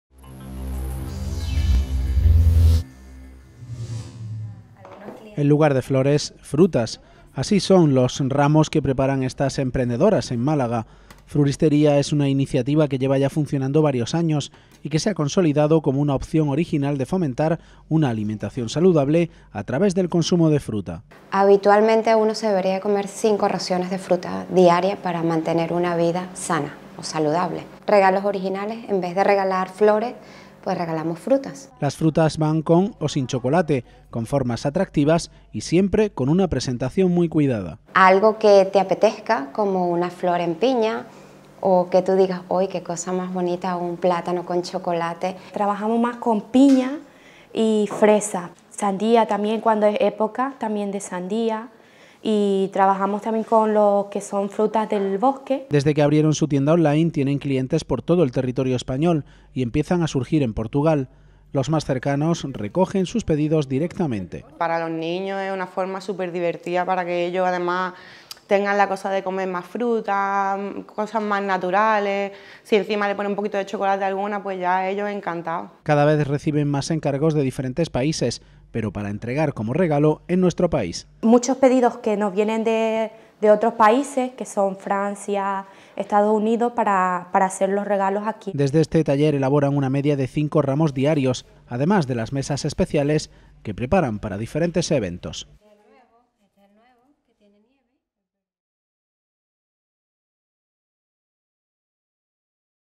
HDL Fruristería: ramos de frutas en lugar de flores para incentivar su consumo diario
Ramos de frutas en lugar de ramos de flores. Aunque a veces, las combinan. Es una iniciativa surgida en Málaga con la que fomentan, de una manera original, el consumo diario de fruta y, también, en celebraciones. Desde Fruristería realizan ya envíos a toda España y reciben encargos de diferentes países.